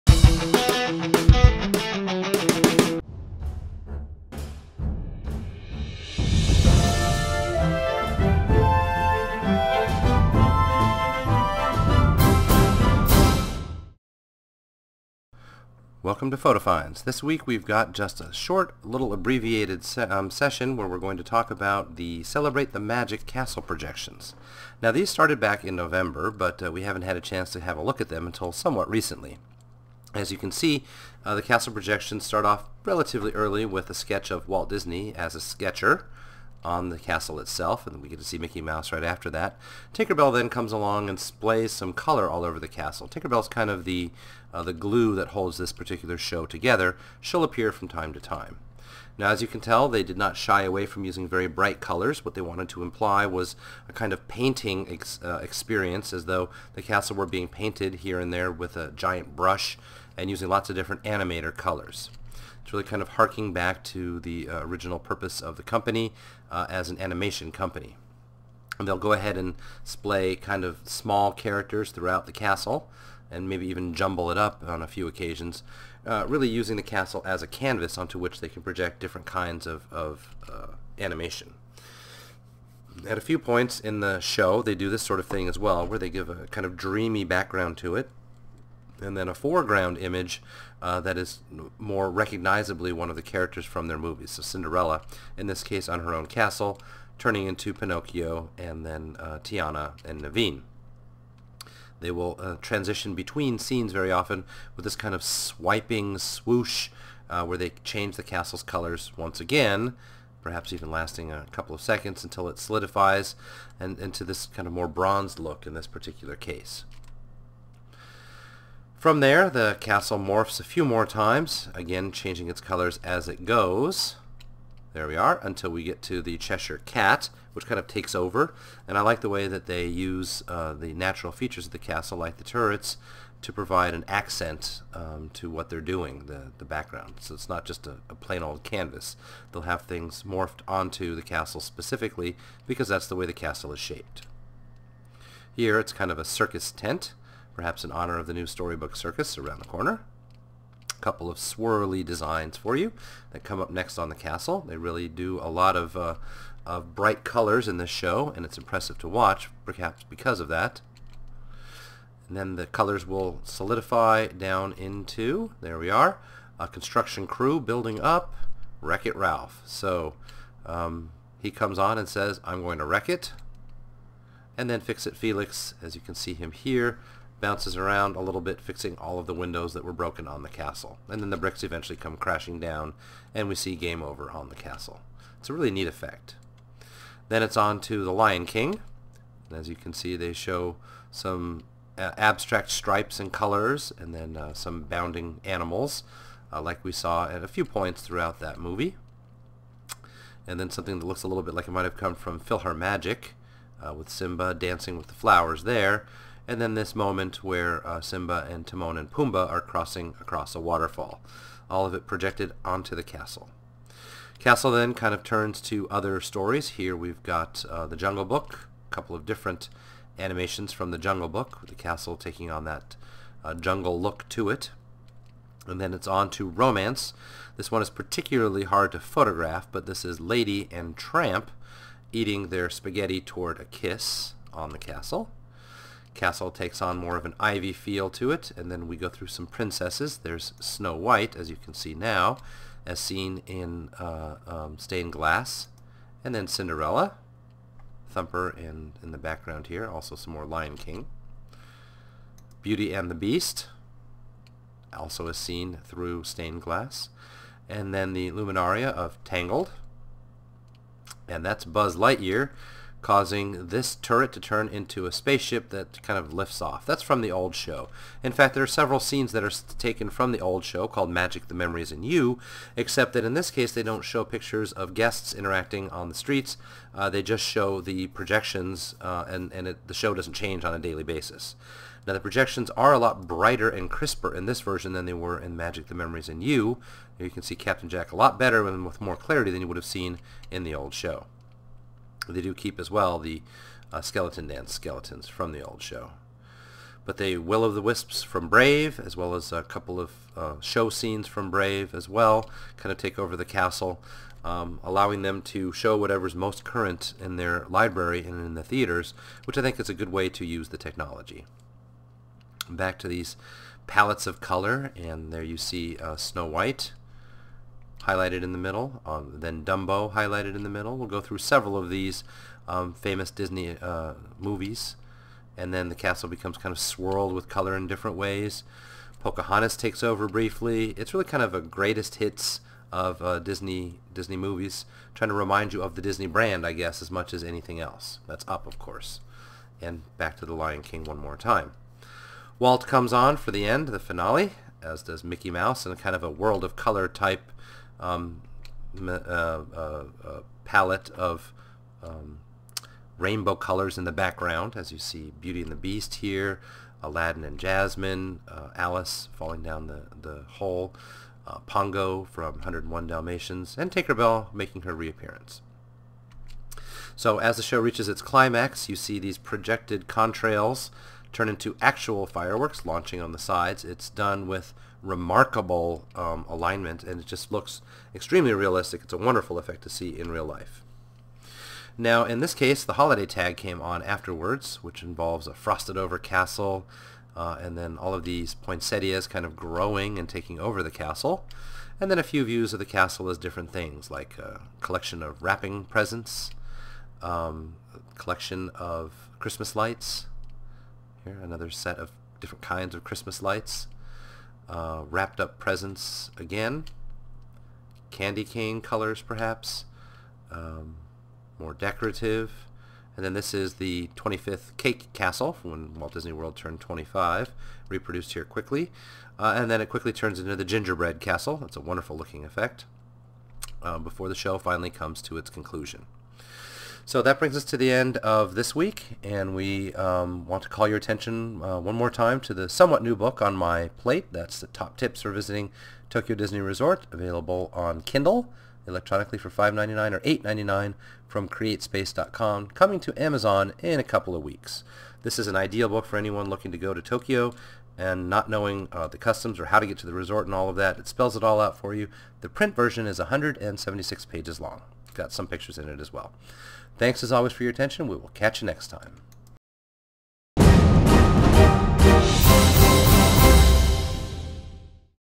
Photo Finds: Celebrate the Magic Castle Projections - Jan. 1 2013
Although they've been around since early November, we haven't had a chance to visit the revised castle projection show until recently. The new show, Celebrate the Magic, continues many themes from the previous show but no longer features guests snapped by PhotoPass. There is a holiday tag at the end with themed projections to match the season.